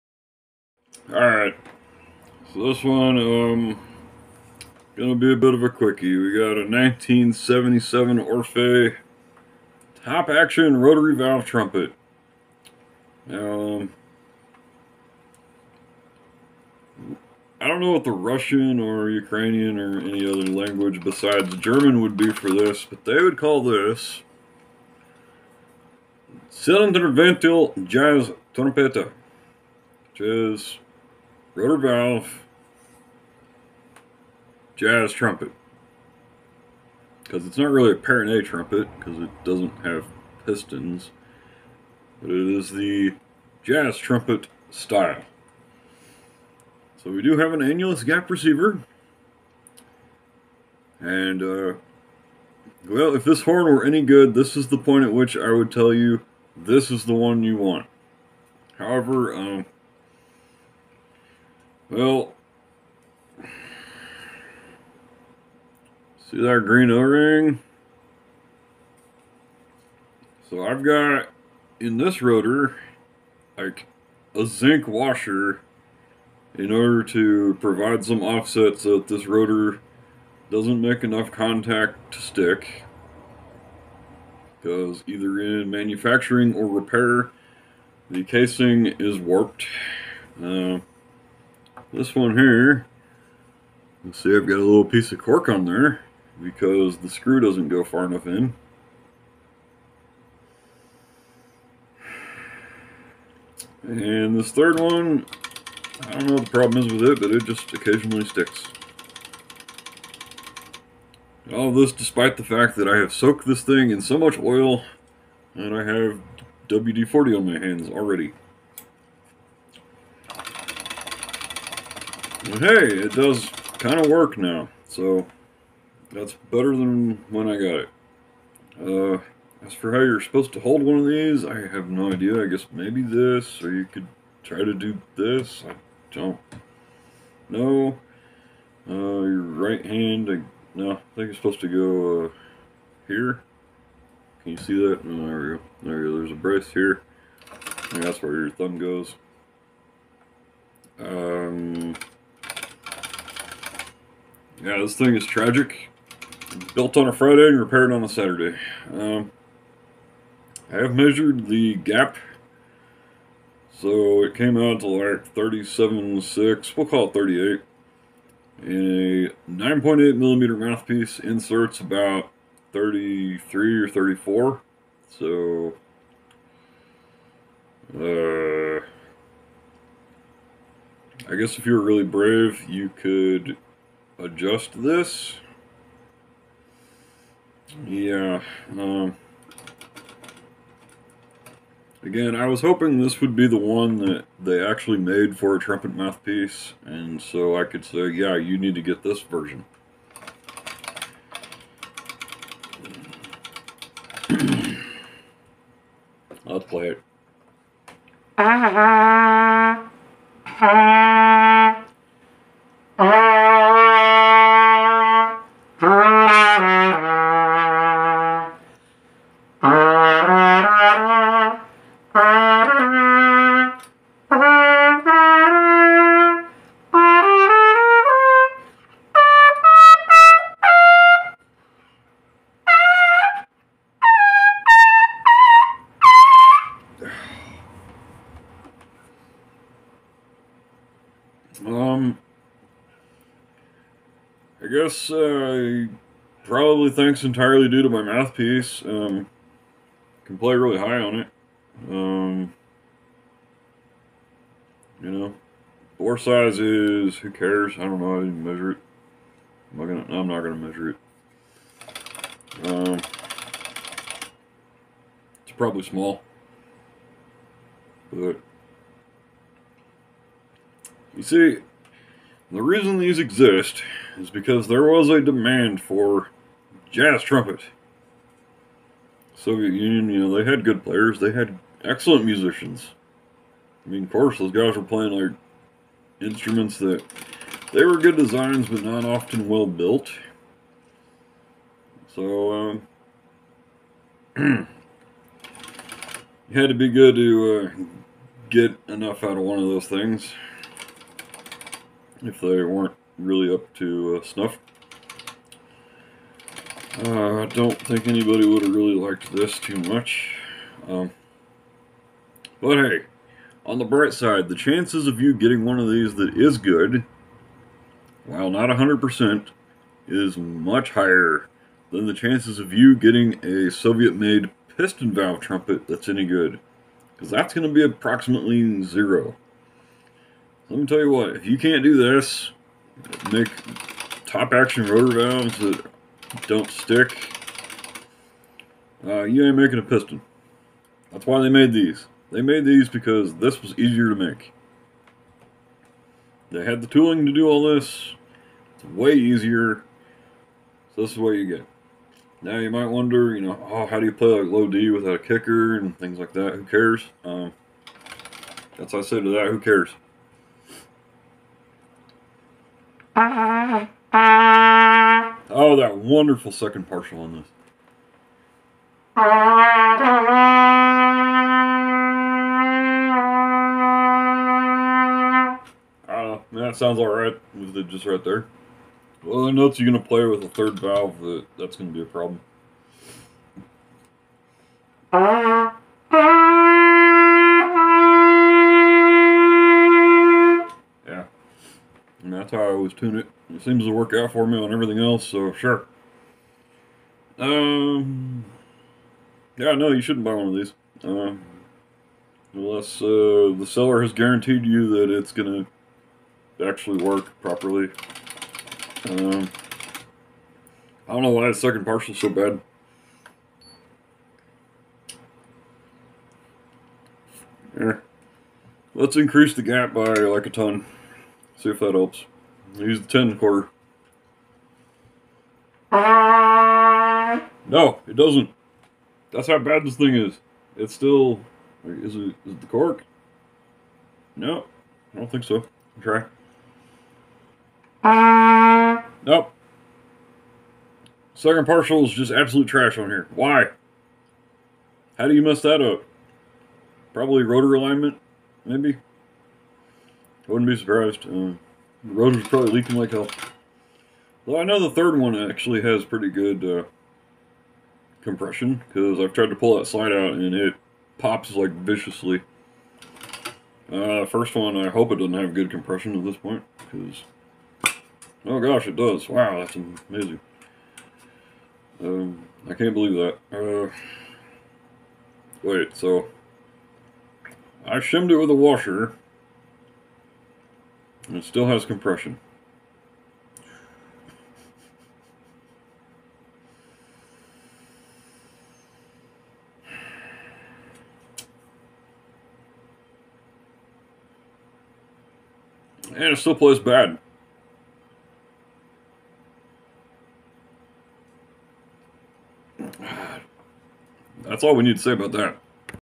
1977 Орфей Trumpet (TARV Jazz Trumpet, Soviet)
I've had some good luck with my Orpheus horns so far.  Until this point...

I suppose that the ol' Zylinderventil Jazz Trompete might have some potential, but I don't think that I'll be bothering to explore that potential because this guy is little more than a beautiful wall hanger.